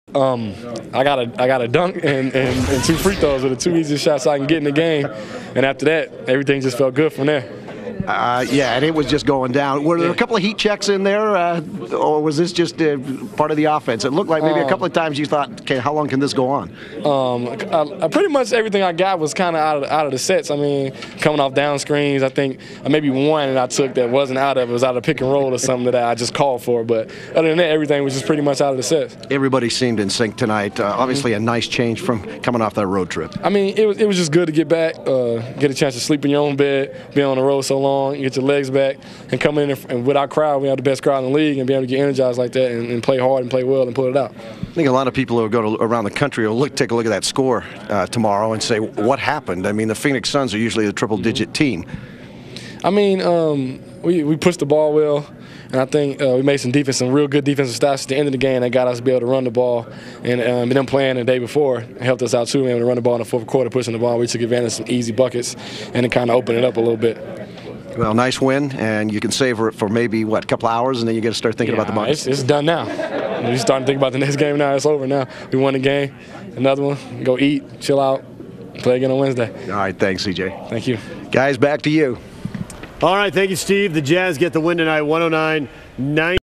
C.J. Miles Interview
KJZZ's interview of C.J. Miles as the Jazz beat the Suns, 109-97.